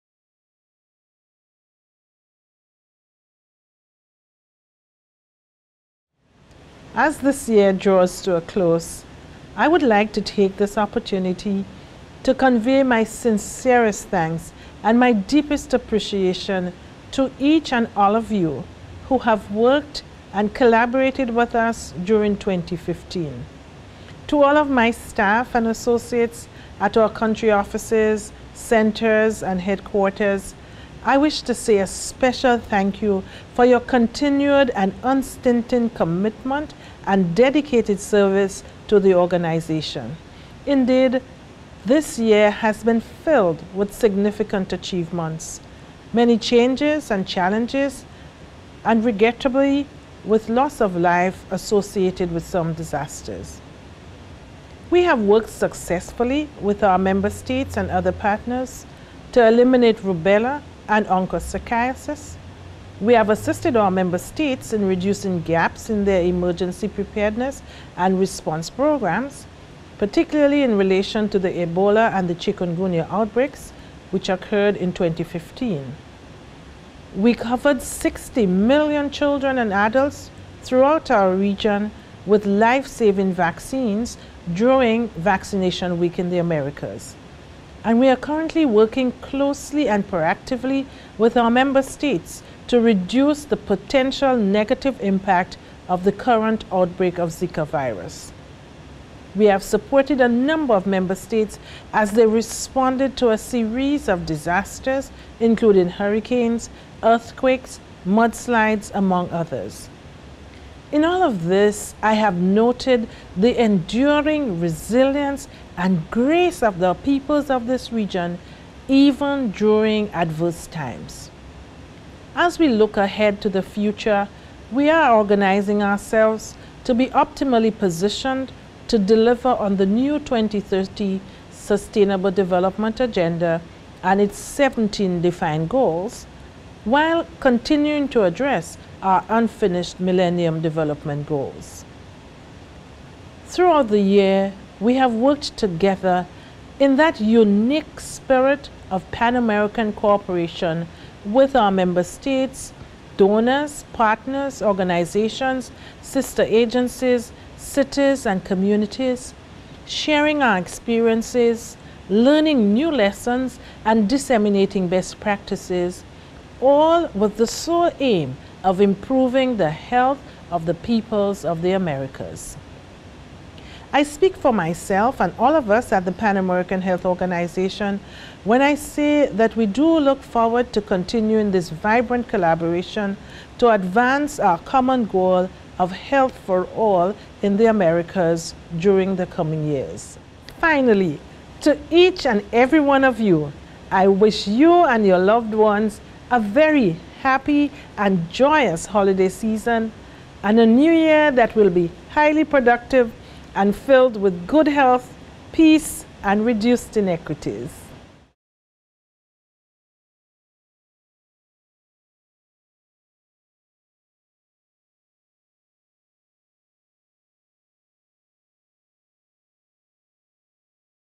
Holiday Message Dr. Carissa F. Etienne - 2015 - PAHO
Holidays Message Dr. Carissa F. Etienne, Director of the Pan American Health Organization PAHO - 2015